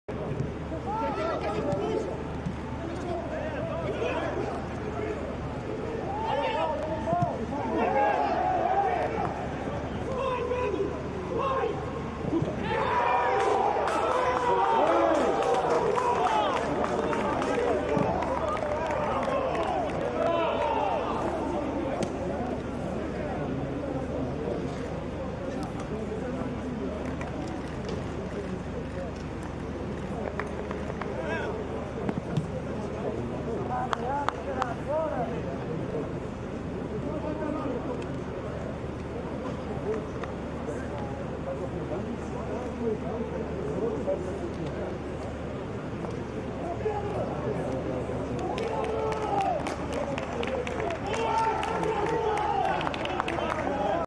Goal | Golo Marques: Belenenses SAD 0-(1) Gil Vicente (Liga 20/21 #29)
Denis sai da sua área para se antecipar a Marques mas acaba por falhar a intercepção, Phete ainda aparece para o corte, mas o jogador gilista consegue atirar para o fundo da baliza vazia.